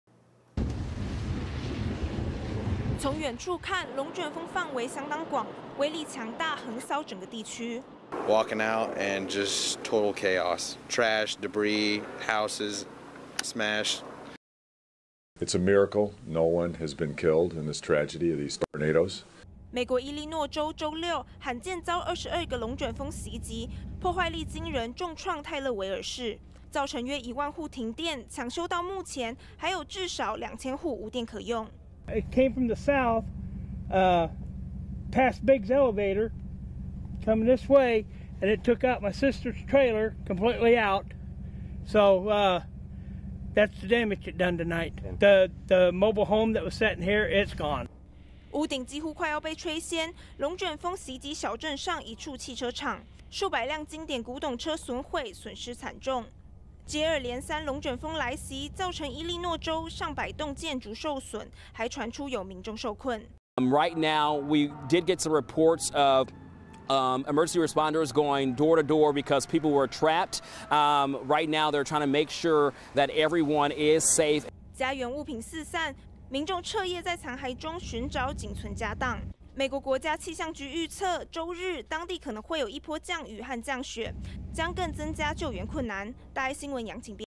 一次來22個！龍捲風襲擊美中西部
來看到全球氣候異常，美國中西部伊利諾州，短短一天之內，連續出現22個龍捲風。強烈破壞力、大規模橫掃，造成超過一百棟建築損毀，大約1萬戶電力中斷。